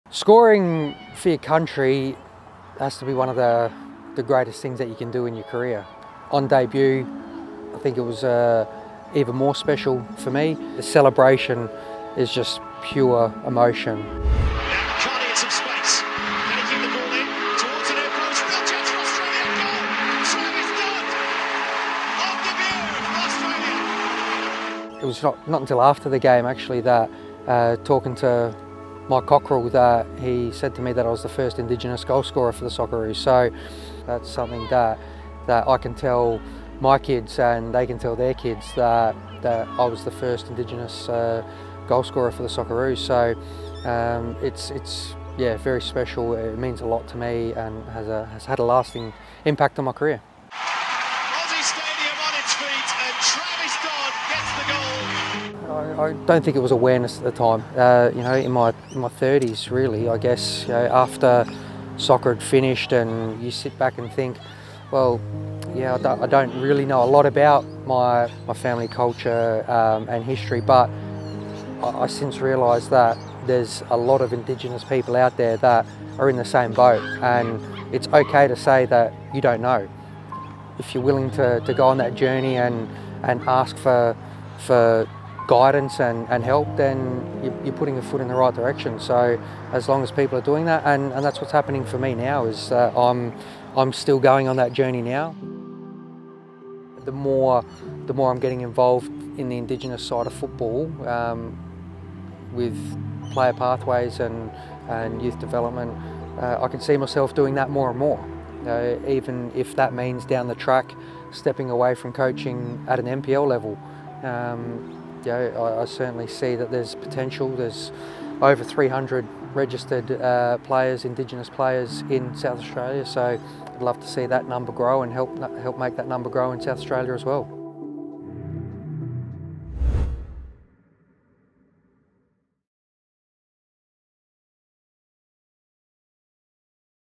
Celebrating NAIDOC Week: Travis Dodd on becoming the Socceroos' first Aboriginal goalscorer
"That's something I can tell my kids, and they can tell their kids" 🙌

As we close out NaidocWeek2022, Travis Dodd powerfully reflects on the moment he became the Socceroos' first Aboriginal goalscorer.